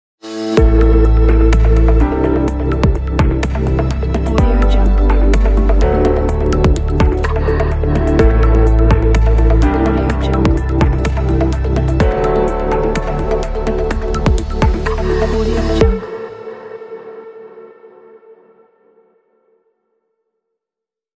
High Technology Logo Opener
This project was created to show your logo and text in the best and unique way. It’s very easy to use – just paste your texts and logo and it’s ready! Of course you have full control over the project because it was created 100% in After Effects. If you have any questions or problems with the project – write to me, I will answer as soon as possible
    Edit everything – all done in After Effect
    Videotutorial included
    Change colors
    Works in all After Effect languages (Expression Universalized)
    Modular Structure (easy to extend for more scenes)